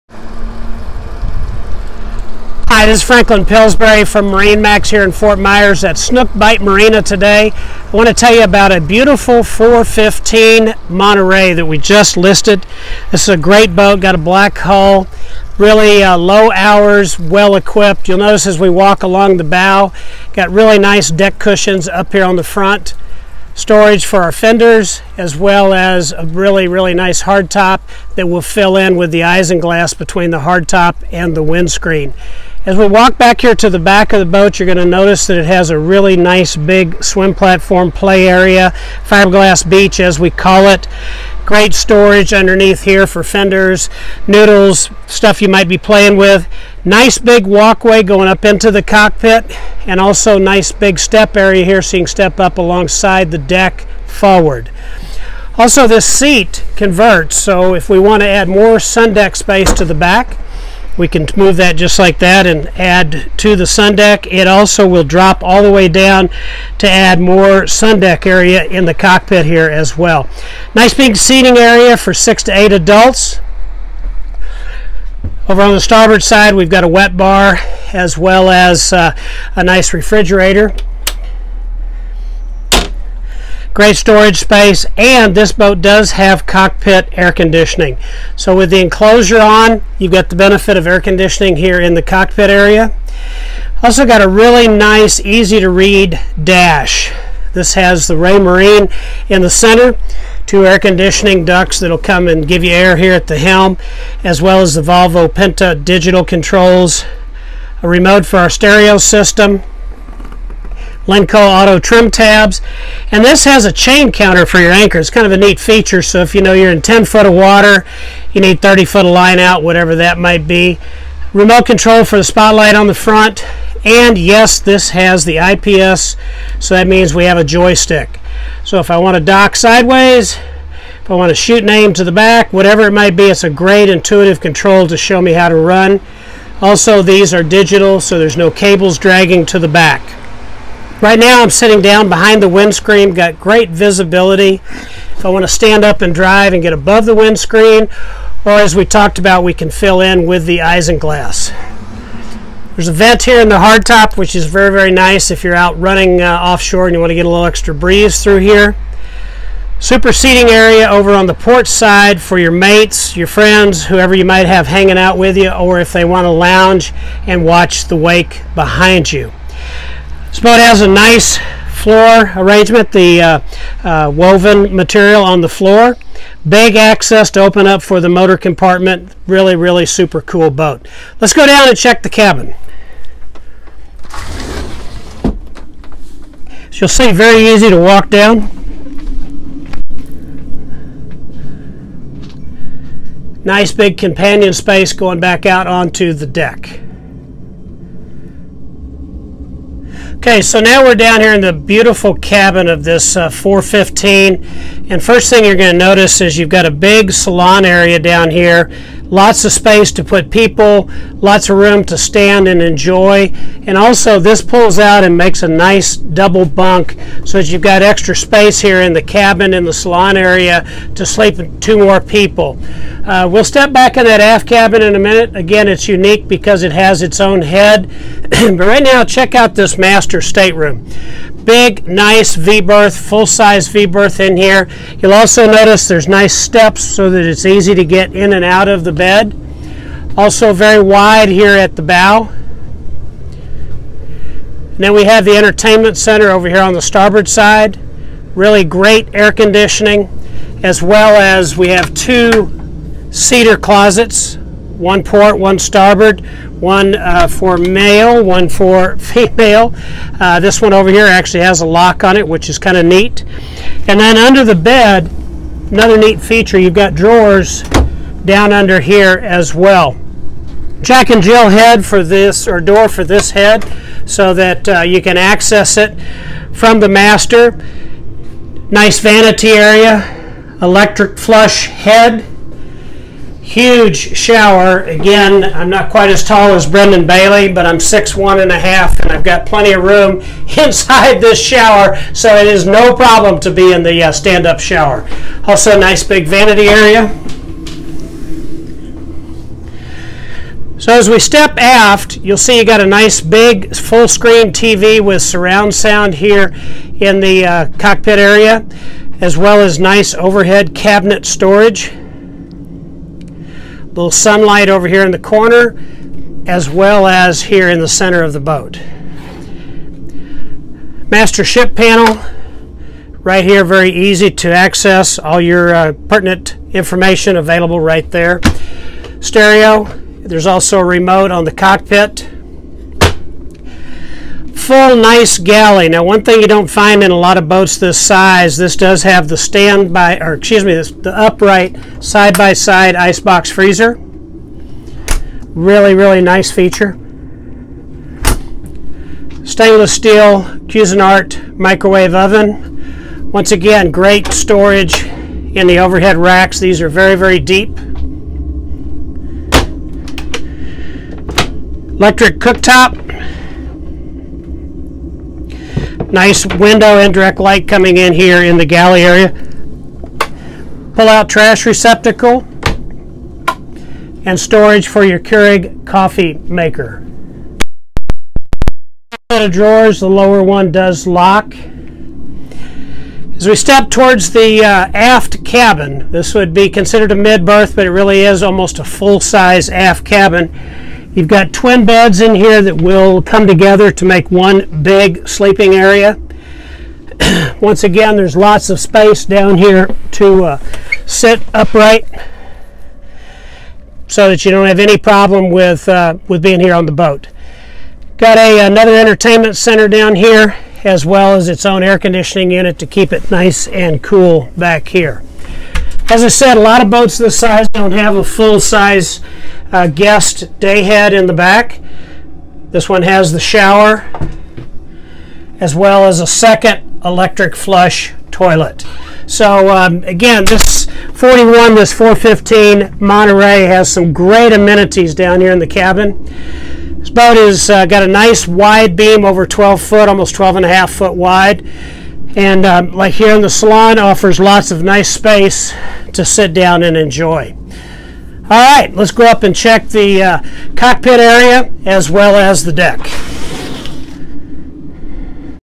2015 Monterey 415 Sport Cruiser For Sale at MarineMax Fort Myers
The Monterey 415 is a smart-looking and well-behaved 40-footer, they managed to over deliver the important features and value in your next Sport Yacht.
The 415 SY is fully designed from the ground up for the Volvo’s IPS pod-drive “Joy Stick” diesel power propulsion system. From the outset Monterey‘s choice of propulsion for the 415 SY is the Volvo IPS. Monterey has led the future of Sport Yacht’s with their performance and handling advantages over conventional inboard power and traditional styled cruisers. MAJOR game-changer for the Sport Yacht owner.
The Monterey 415 SY features A two-stateroom, and a two-head design, most express boats this size have a single head! With the 415 Sport Yacht’s luxurious cherry wood trim, wood flooring, posh Ultra leather seating, and designer furnishings, the accommodations aboard the 415 SY are better than that of the best European imports.
The single berths in the aft mid cabin easily convert to a double with a filler, and a home theater system with flat-screen TV is well placed in the salon. In the cockpit, a unique foldout table deploys from the aft portside lounge providing an alternative to the below deck dinette on warm summer evenings.
A hardtop, cockpit wet bar, and windlass make “Being on the HOOK” a great afternoon of relaxing and playing in the water. With a large lounge/sun pad on the aft, bilge/engine access is as good as it gets in a boat of this type. Joystick controls make low-speed control easy. Cruise at 26–28 knots (35 top) with the Volvo IPS diesels. This 415 SY Monterey is ready to take you on the BOATING Experience of a lifetime!